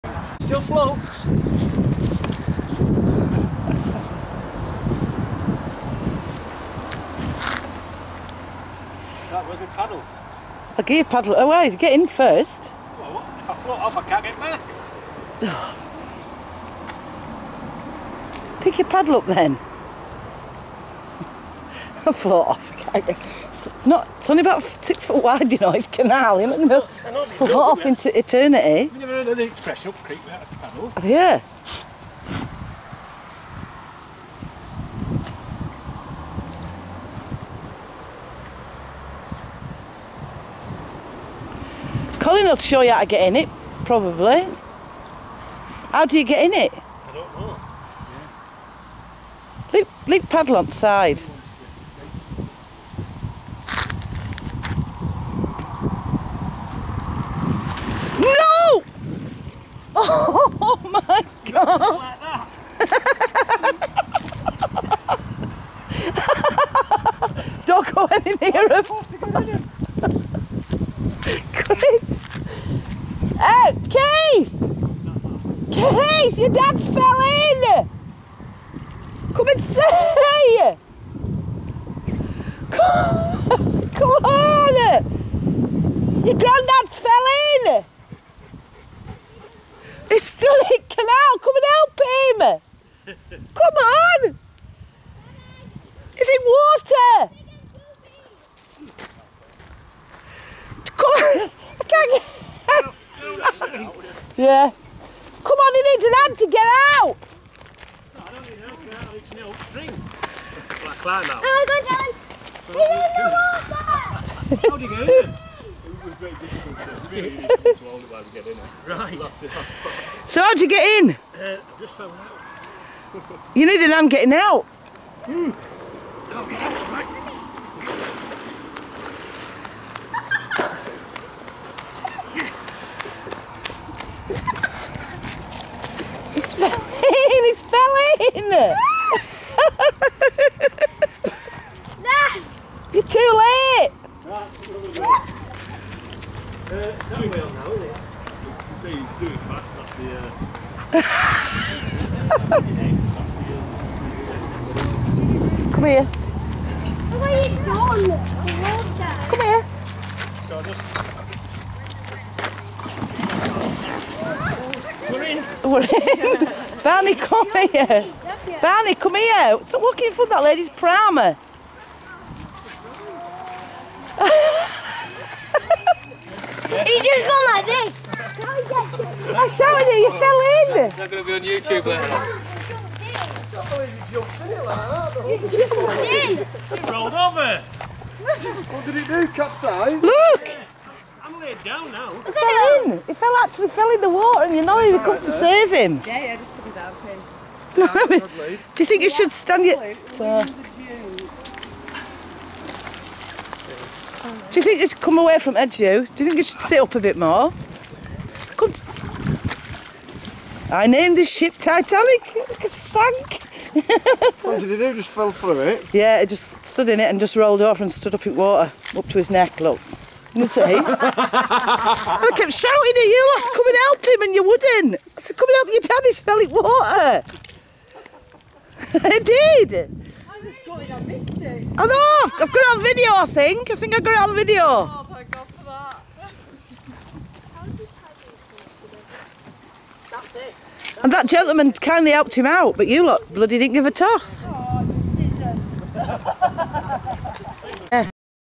Canoe.m2ts
Grandad saw a friend in an inflatable canoe and he thought he would buy one. This is his first attempt ever at canoeing. How does he get in? how does he get out? Crazy Grandad. This was rejected by "You'v been framed"